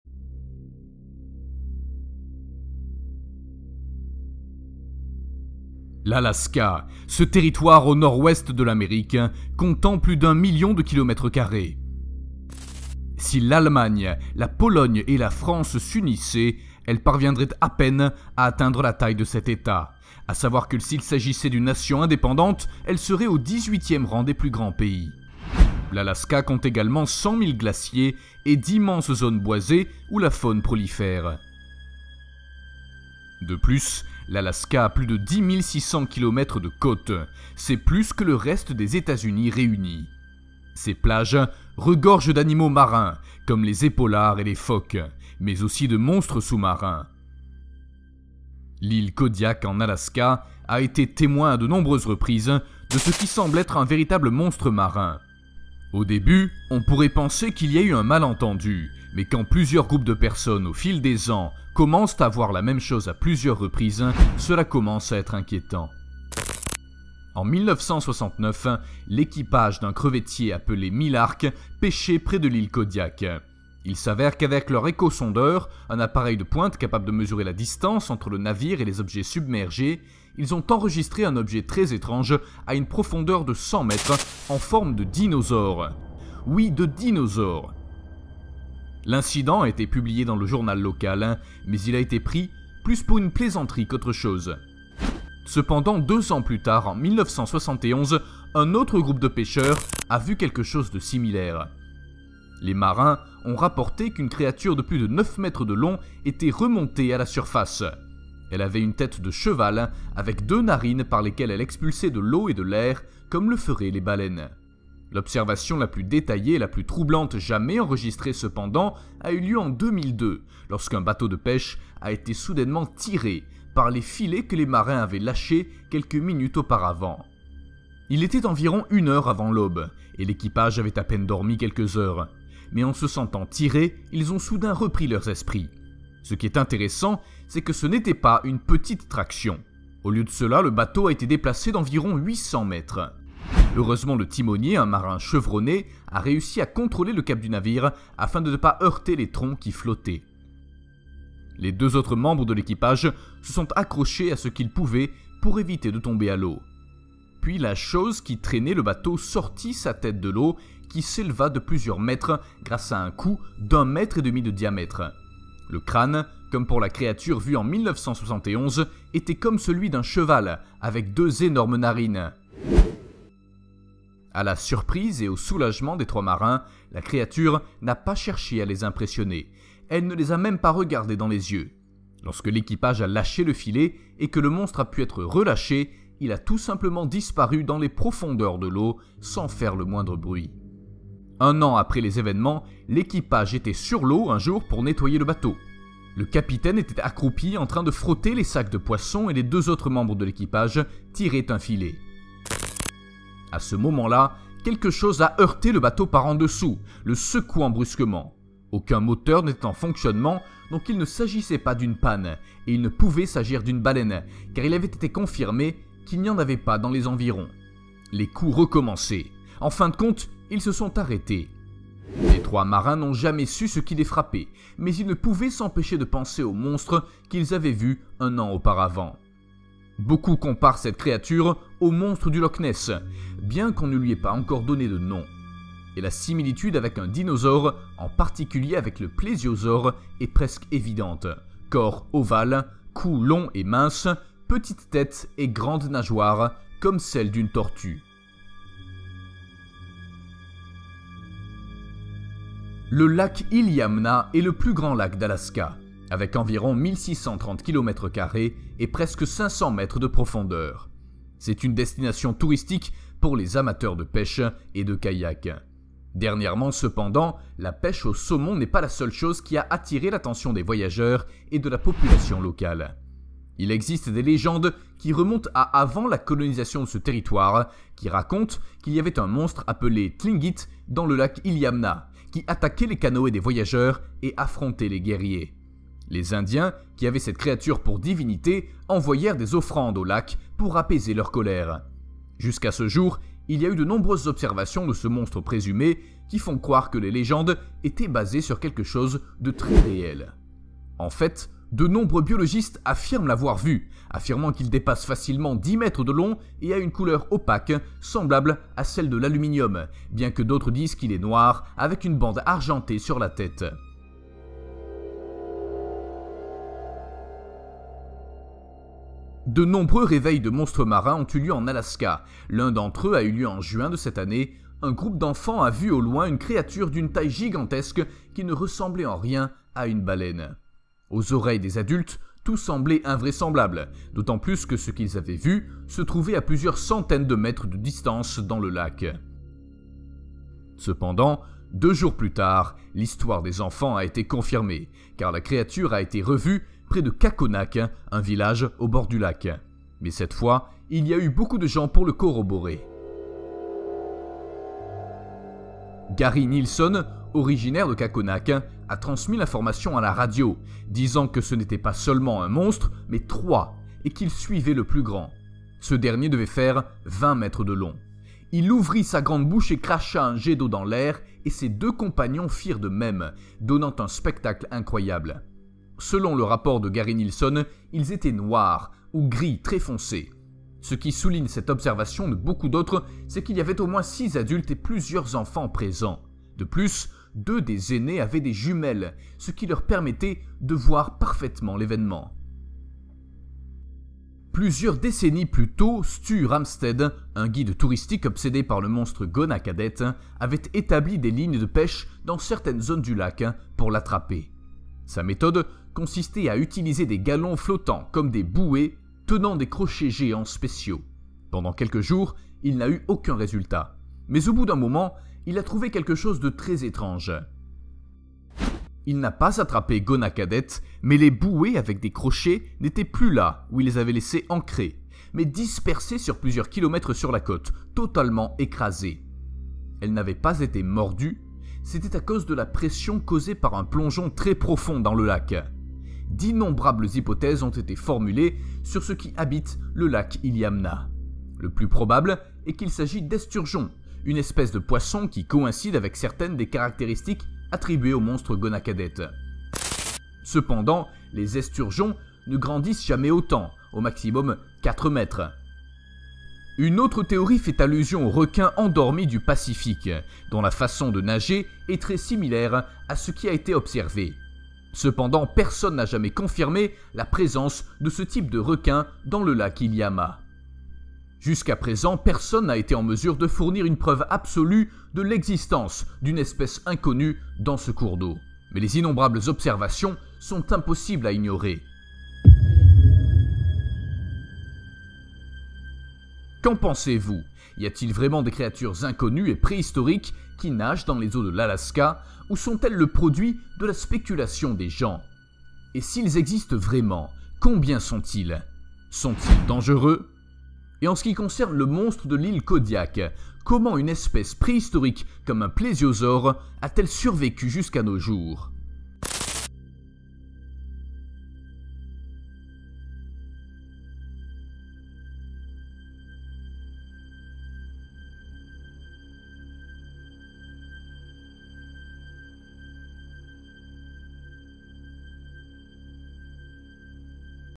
La Terreur Sous les Eaux de l'Alaska.
L’Alaska : ce territoire au nord-ouest de l'Amérique, comptant plus d'un million de kilomètres carrés. Si l'Allemagne, la Pologne et la France s'unissaient, elles parviendraient à peine à atteindre la taille de cet État, à savoir que s'il s'agissait d'une nation indépendante, elle serait au 18e rang des plus grands pays.
L'Alaska compte également 100 000 glaciers et d'immenses zones boisées où la faune prolifère.
De plus, l'Alaska a plus de 10 600 kilomètres de côtes ; c'est plus que le reste des États-Unis réunis. Ces plages regorgent d'animaux marins, comme les épaulards et les phoques, mais aussi de monstres sous-marins.

🎬Videos Similaires.